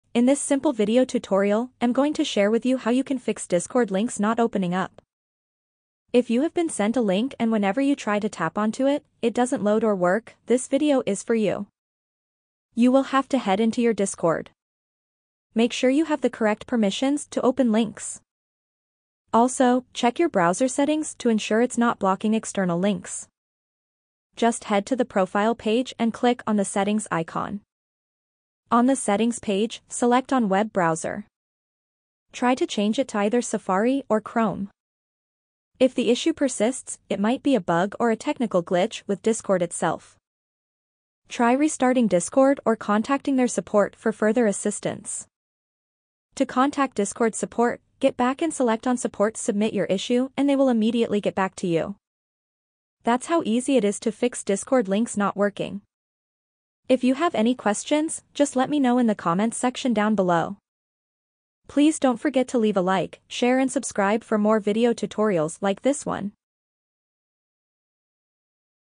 Fix Links Not Working On Discord | Discord Links Not Opening Up | Problem Solved!
In this Simple video tutorial, am going to share with you how you can fix discord links not opening up. If you have been sent a link and whenever you try to tap onto it, it doesn’t load or work, this video is for you. You will have to head into your discord. Make sure you have the correct permissions to open links. Also, check your browser settings to ensure it's not blocking external links.

 Fix discord links not  working.